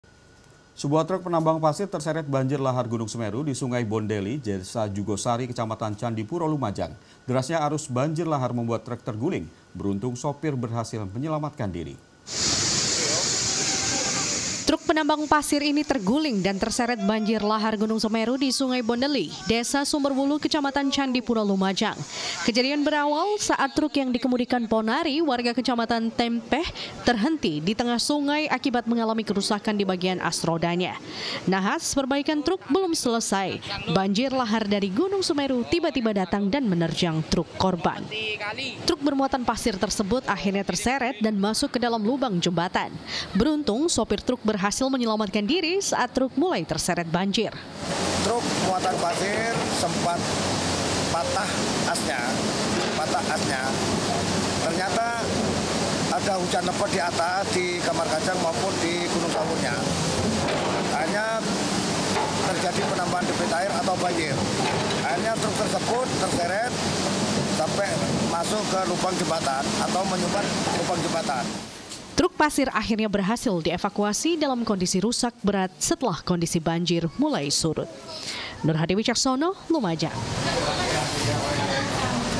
Nekat Ambil Pasir, Truk Dihantam Banjir Lahar
Sebuah Truk Penambang Pasir Terseret Banjir Lahar Gunung Semeru Di Sungai Bondeli Desa Jugosari  Kecamatan Candipuro Lumajang Minggu Petang . Derasnya Arus Banjir Lahar Membuat Truk Terguling Dan Terendam Banjir Lahar . Beruntung Sopir Truk Berhasil Menyelamatan Diri . Sementara Itu  Truk Pasir Mengalami Kerusakan Parah .